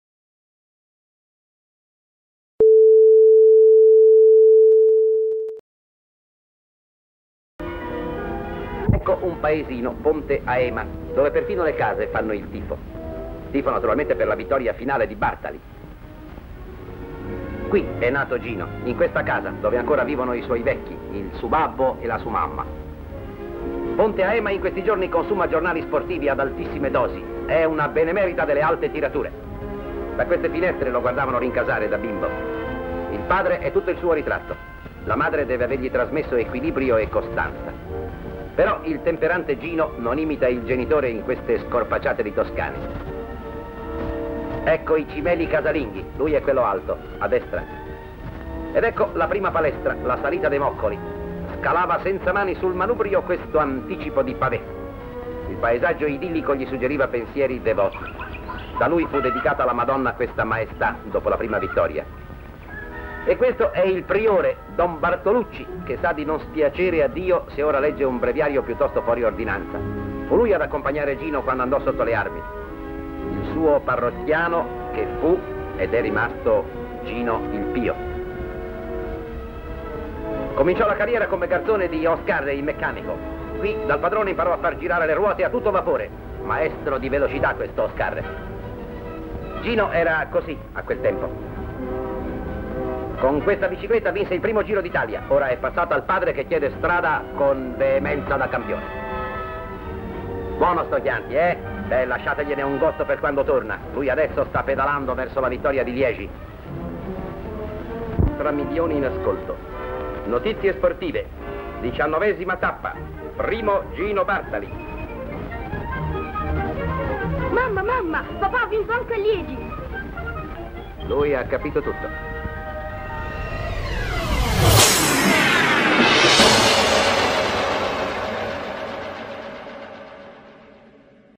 Ponte a Ema (FI) Paese natale di Gino Bartali HD
Ponte a Ema è un piccolo centro toscano ai piedi della collina della Fattucchia, lungo Via Chiantigiana. In parte appartiene al comune di Firenze (Quartiere 3 Gavinana-Galluzzo), una parte appartiene al comune di Bagno a Ripoli; fa confine tra i due comuni il corso stesso del fiume Ema, attraversato dallo storico ponte. In corrispondenza del ponte, sulle due sponde del fiume si è formato il borgo storico di Ponte a Ema.
Paese natale di Gino Bartali (e del fratello Giulio), ospita oggi il Museo del ciclismo Gino Bartali a lui dedicato. A Ponte a Ema è presente dal 1872 una "Casa del Popolo". Tratto dagli archivi Mauro Montagni.  Riversamento e digitalizzazione Dr. Fernando Menichini